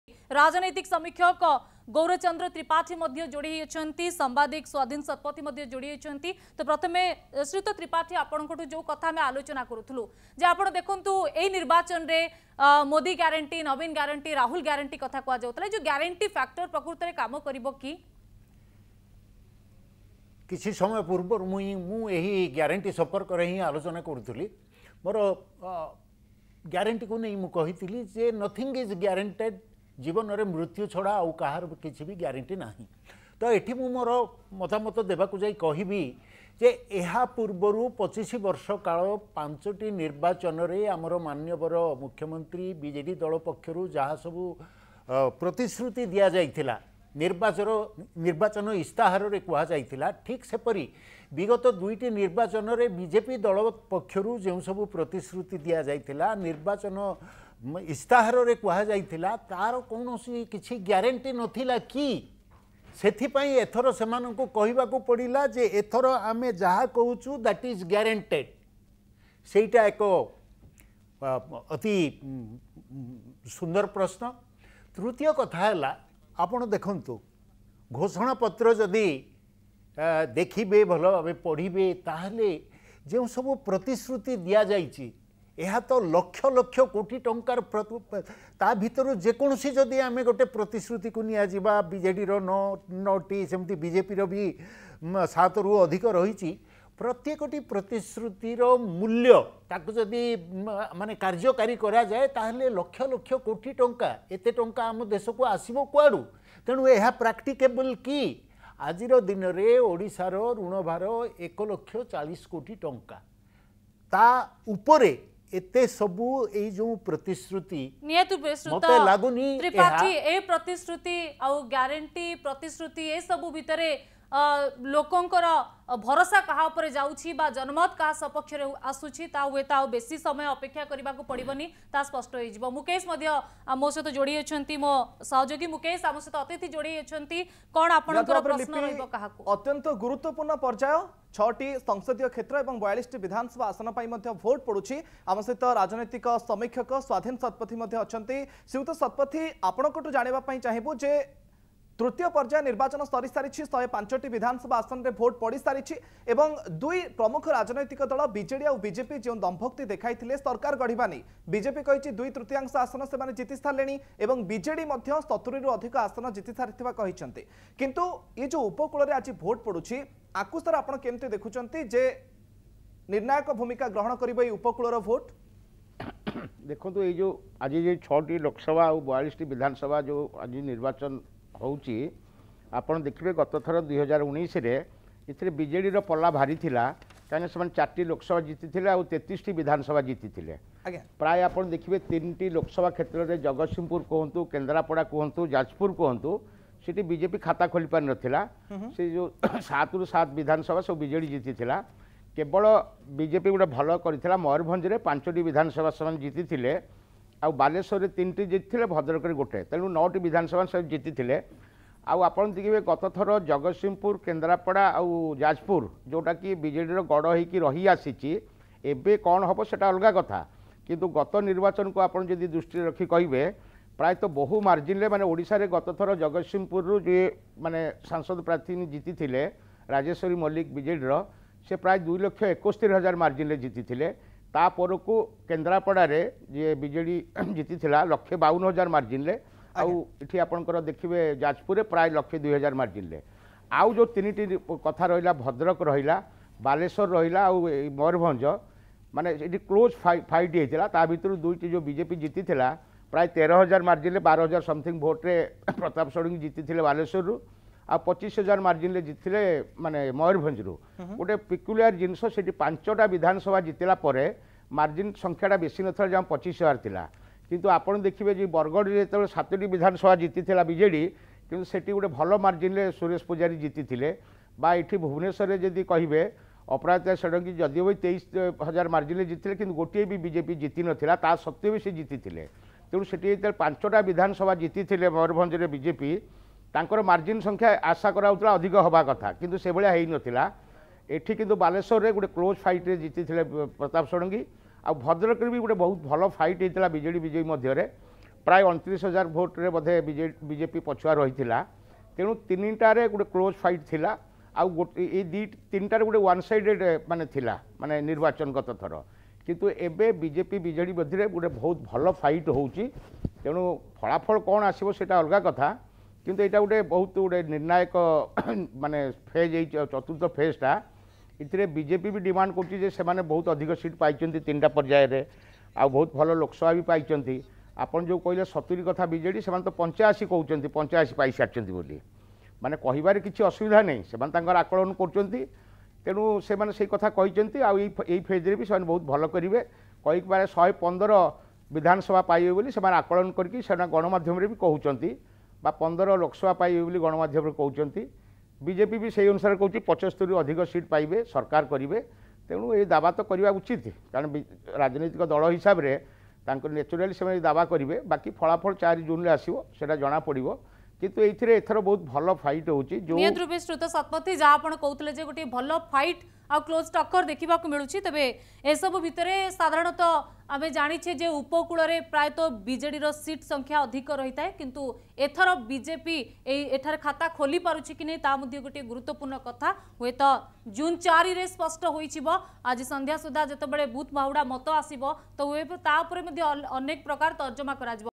Will Modi And Naveen Guarantee Lure Voters In Odisha's 4th Phase Polling?
Explore the potential impact of Prime Minister Modi and Chief Minister Naveen Patnaik in attracting voters during Odisha's 4th phase polling. Delve into the strategies and appeals made by these leaders to sway voters. Stay informed about the dynamics shaping voter behavior in Odisha..